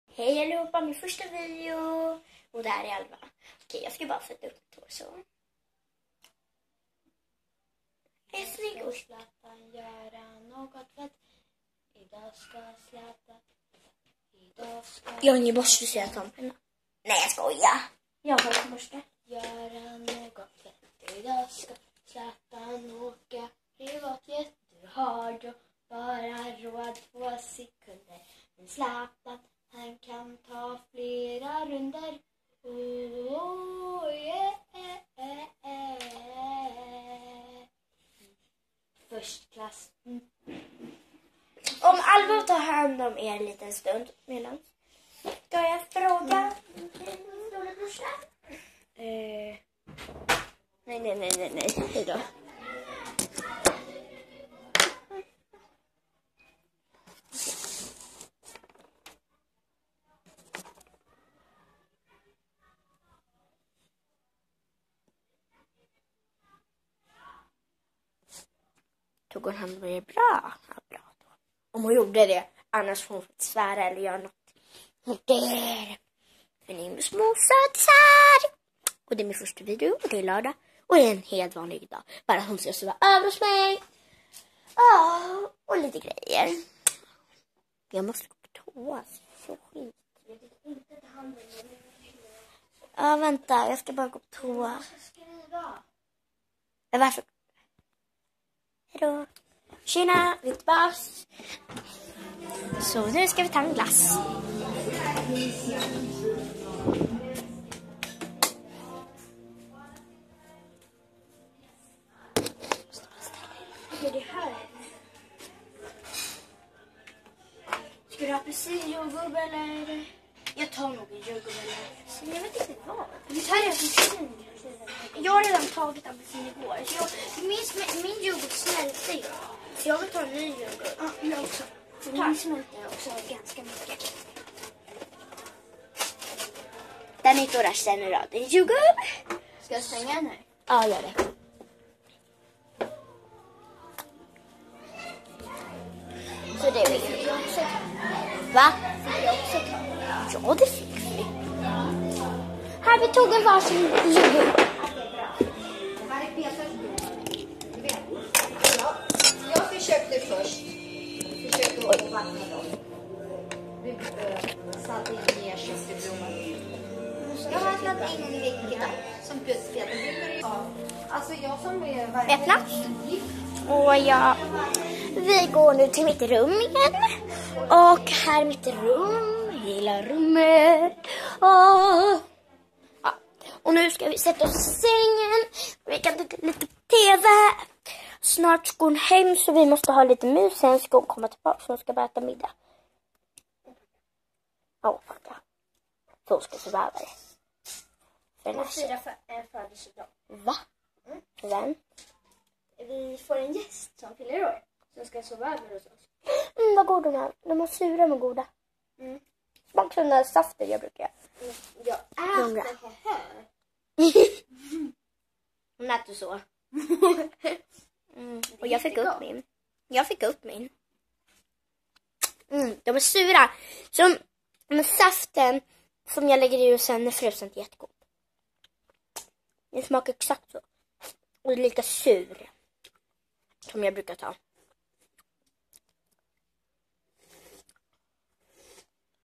Alva är hemma hos mig 🎃💝🐱😊
Alva kommer sova över hos mig 😊😀😍